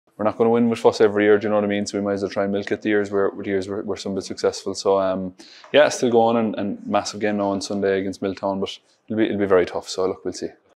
2023 GAA ALL STARS - DAVID CLIFFORD LOOKING FORWARD TO SINKING HIS TEETH INTO MILLTOWN THIS WEEKEND
FOOTBALLER OF THE YEAR - FOSSA V MILLTOWN